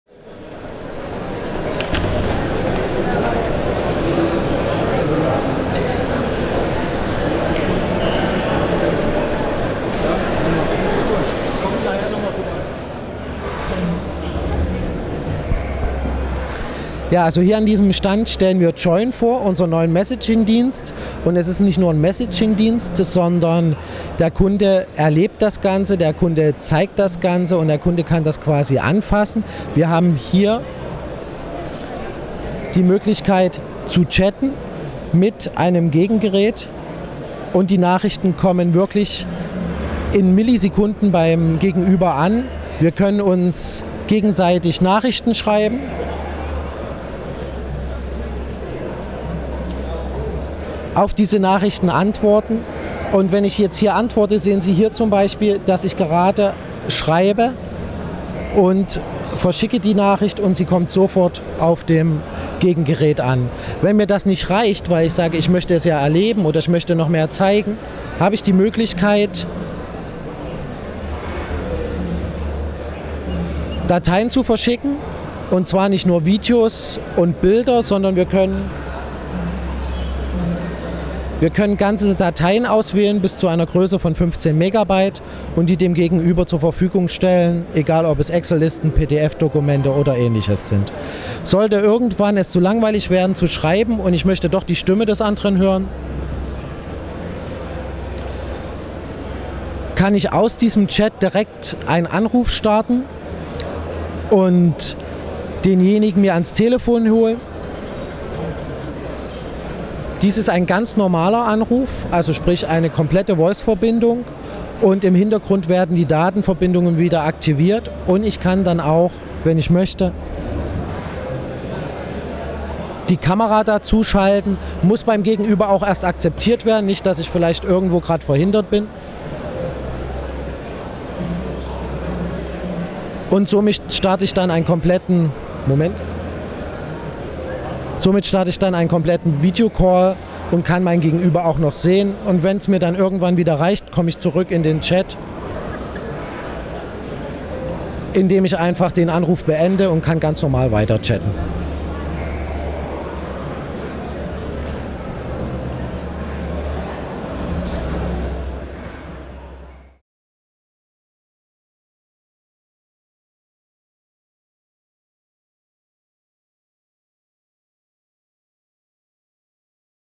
joyn - neuer anbieterübergreifende Nachrichtendienst: Vorstellung auf Telekom Stand IFA 2012
. joyn ist der neue Nachrichtenstandard und gilt als der SMS-Nachfolger. Aber joyn bei Telekom und anderen Anbieter wie o2 und Vodafone kann mehr, also nur SMS. So können Sie mit joyn Dateien aller Art versenden, auch direkt während eines Telefonates und auch Videotelefonie ist mit joyn kein Problem. In diesem HD-Video stellen wir Ihnen joyn vor, das Video entstand am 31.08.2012 auf dem IFA Stand der Telekom.